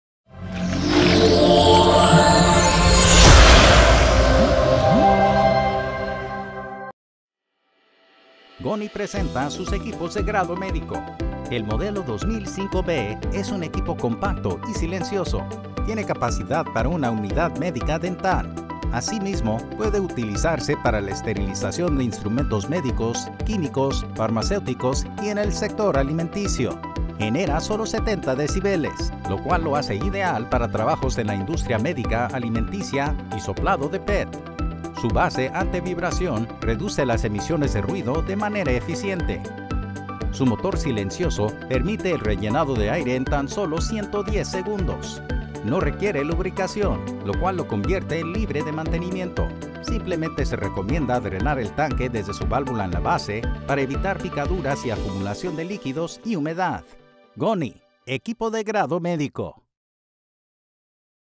Compresor silencioso de 2 HP y 50 lt libre de aceite Modelo 2005B
Equipo compacto libre de aceite, con un tiempo de rellenado de tan solo 110 segundos. Al ser un sistema silencioso emite únicamente 70 decibeles para disminuir el ruido, la base del motor posee anti-vibración en cada pata, además de ser libre de mantenimiento. Están exentos de cualquier partícula de aceite, lo que los convierte en el  compresor ideal para la industria médica, alimenticia y de soplado Pet, entre otras.